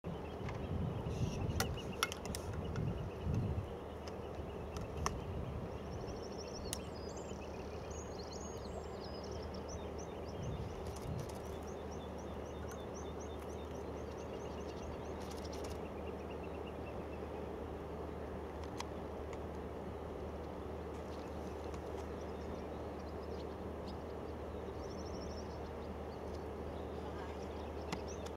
Corncrake: Tory Island, Donegal, Ireland. May 2016.
Tory Island is one of the best places to see corncrakes in Ireland. On this occasion there were five or six daytime calling males at scattered locations around the island. The last week of April to mid-May is the best time to see them before the vegetation too dense, though it is possible to see them after this happens. The males can be heard calling in the evening/night later into the Summer.